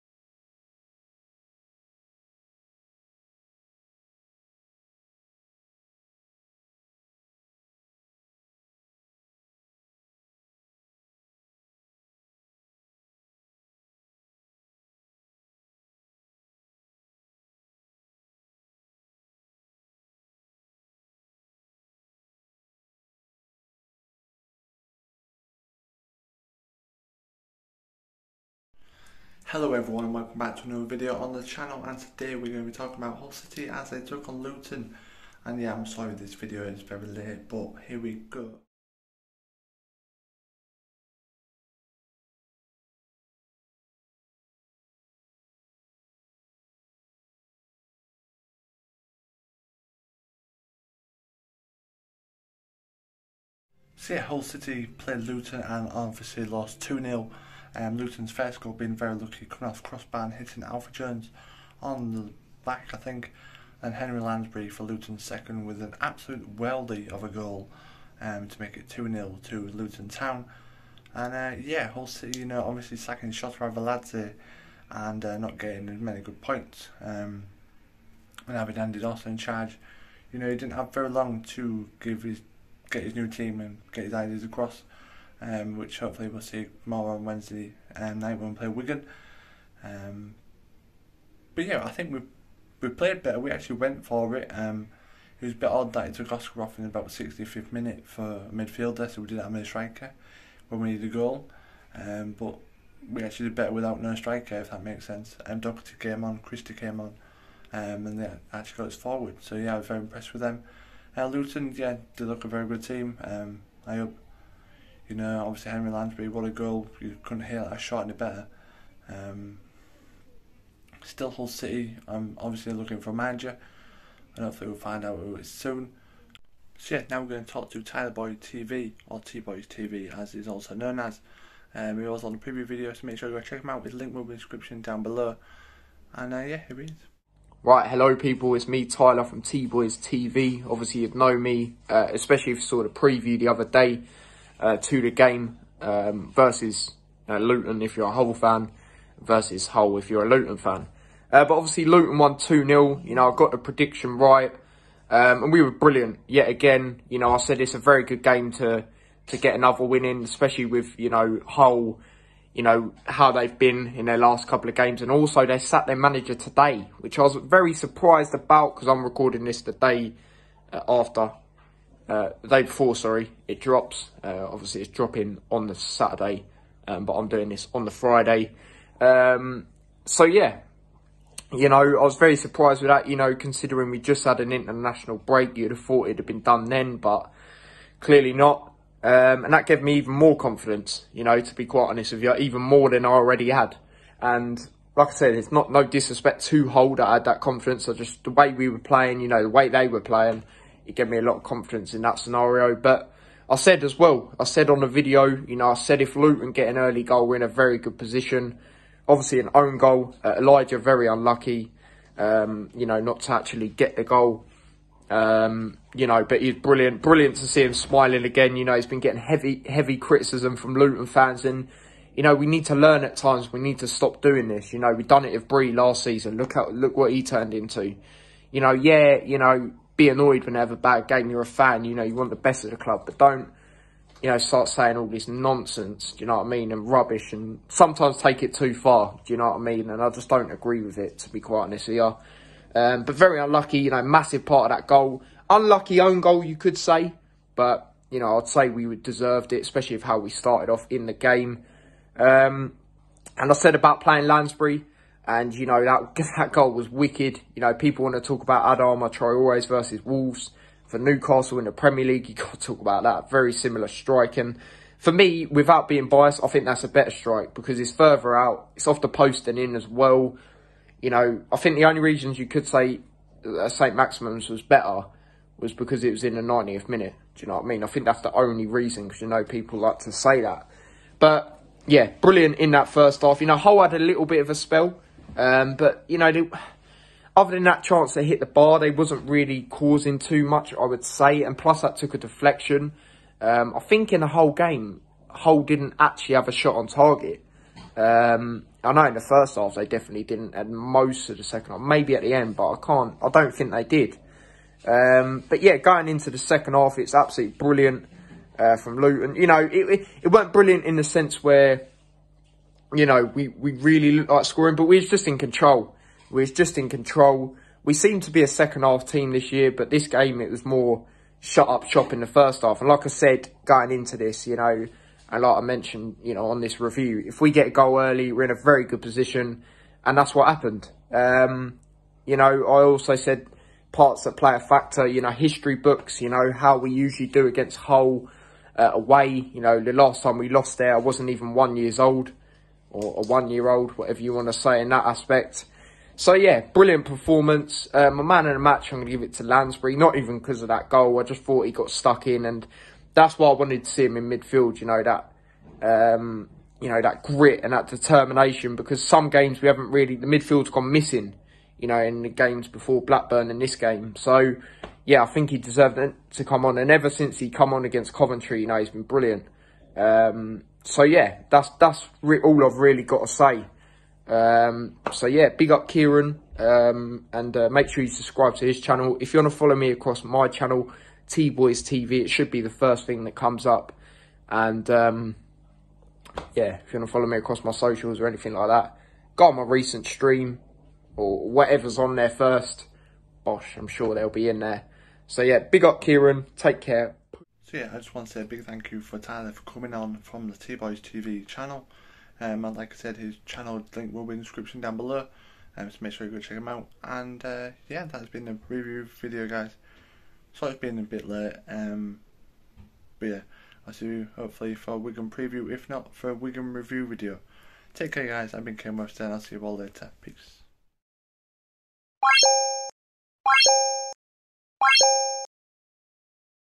HULL STILL LOSING - HULL CITY VS LUTON TOWN REVIEW
Match 11: Hull City Vs Luton Town

Join me as I review the game between Hull City and Luton Town

I am joined by Luton Fan Tyler